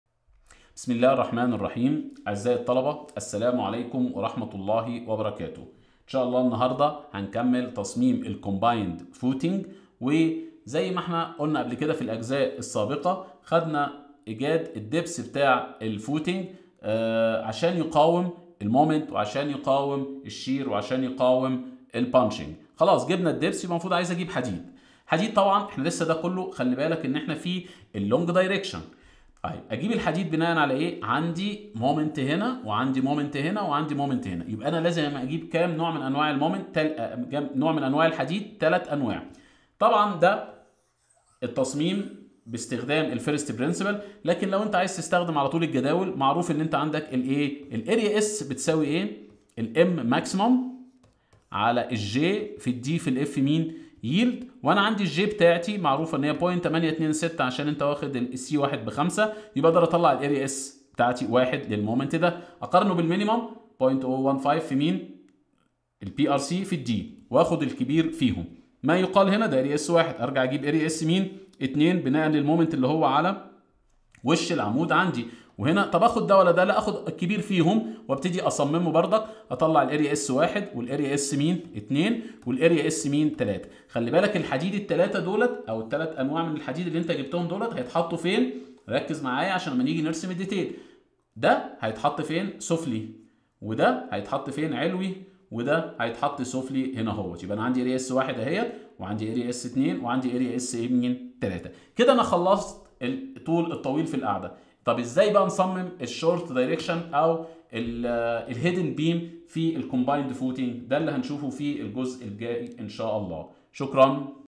Foundations Engineering 2 | C3-L7 | Combined Footing subjected to moment - 7
Chapter 3 : Design of combined footing subjected to eccentric forces
Lecture 7 :  Combined Footing subjected to moment - 7
Explain the Design of Combined Footing subjected to moment steps (calculate the area of steel in the long direction)